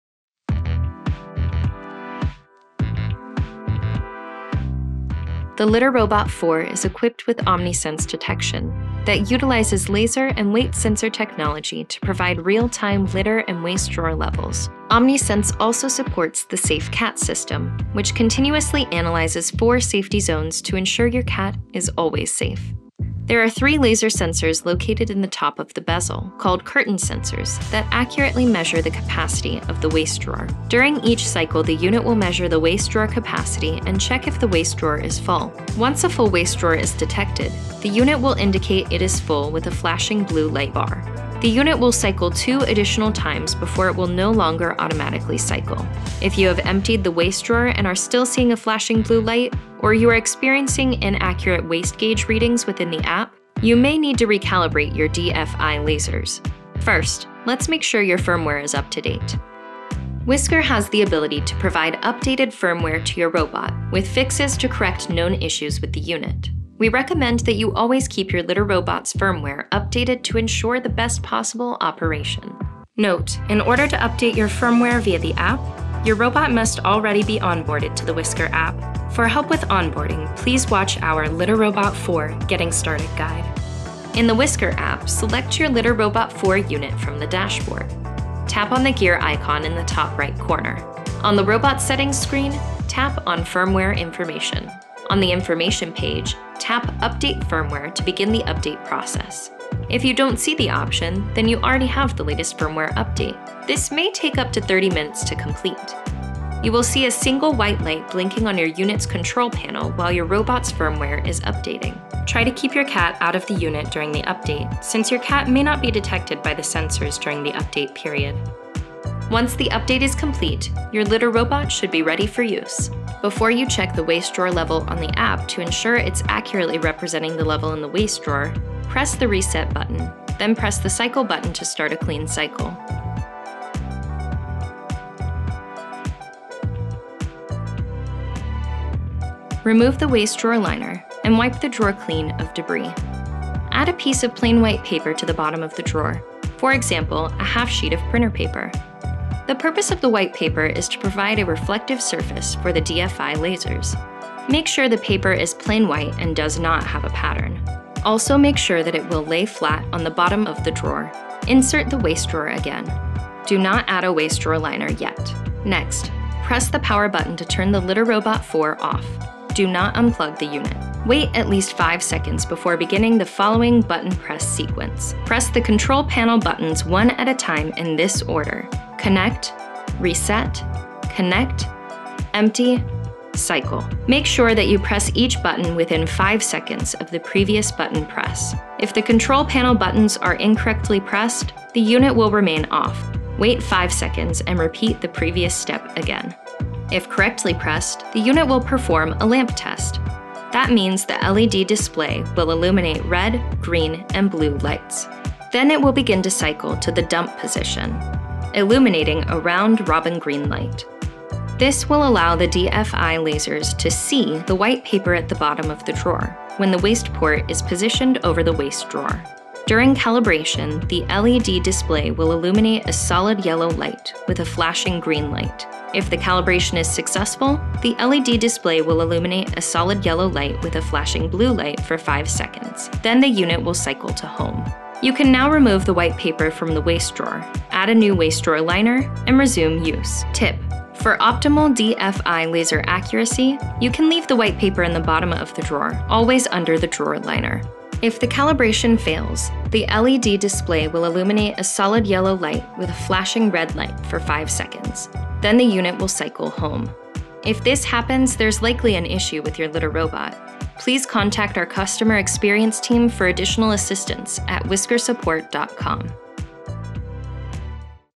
How to calibrate the OmniSense Laser Sensors | Litter-Robot 4
The Litter-Robot 4 is equipped with OmniSense™ detection, which utilizes laser and weight sensor technology to provide real-time litter and waste drawer levels. Omnisense™ also supports the SafeCat system, which continuously analyzes four safety zones to ensure your cat is always safe.

There are 3 laser sensors located in the top of the bezel, called curtain sensors, that accurately measure the capacity of the waste drawer.

During each cycle, the unit will measure the waste drawer capacity and check if the waste drawer is full. Once a full waste drawer is detected, the unit will indicate it is full with a Flashing Blue light bar.

The unit will cycle (2) additional times before it will no longer automatically cycle.

If you have emptied the waste drawer and are still seeing a flashing blue light or you are experiencing inaccurate Waste Gauge readings within the app, you may need to recalibrate your OmniSense™ laser sensors. Please follow the steps outlined in this video.

Consult our related support articles or contact our team.
🛠️ Support Articles:
- Litter-Robot 4: Calibrating the drawer full indicator

🕒 Timestamps:
00:00 Introduction
00:15 SafeCat System
00:35 Flashing Blue Light - Full Waste Drawer
00:56 Firmware Update
01:24 Use Whisker App
02:01 Ready for use
02:14 Calibrating the Sensors
03:54 Remove Paper and Resume Use
04:07 Contact support